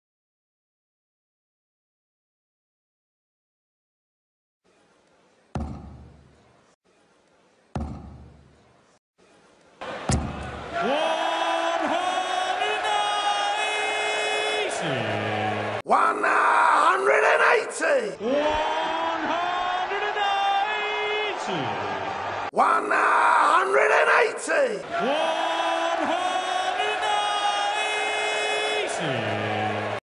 Qneusje Hans en zijn 180-er
Ons "Qneusje" Hans kon natuurlijk niet achter blijven. Hij gooide in Waalre zijn 834e 180-er, maar alleen van deze zijn beelden. KLASSE...